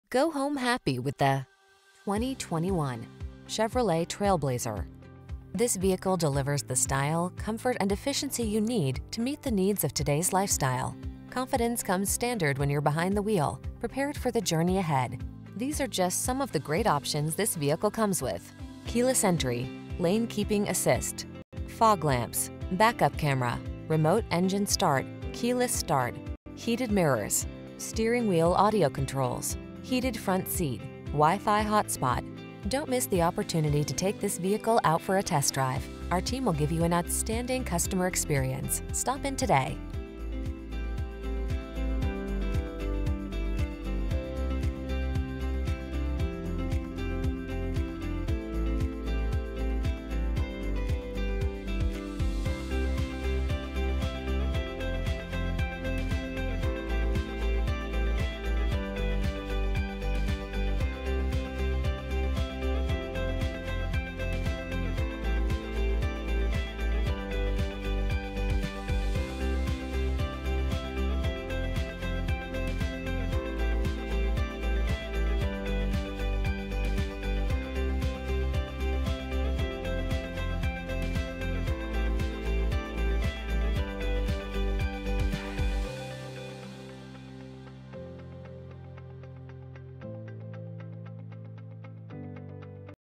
2021 Chevrolet Trailblazer Worthington, Columbus, Westerville, Powell, Delaware XF1T117031
Mosaic Black Metallic New 2021 Chevrolet Trailblazer available in Worthington, Ohio at Mark Wahlberg Chevrolet of Worthington. Servicing the Westerville, Powell, Delaware area.

700 E. Dublin-Granville Road

Mosaic Black Metallic 2021 Chevrolet TrailBlazer ACTIV FWD CVT 1.3L Ecotec Turbo DOHC SIDI w/VVT29/33 City/Highway MPG
LPO  ALL-WEATHER FLOOR MATS,AUDIO SYSTEM  CHEVROLET INFOTAINMENT 3 SYSTEM  7 diagonal color touchscreen  AM/FM stereo.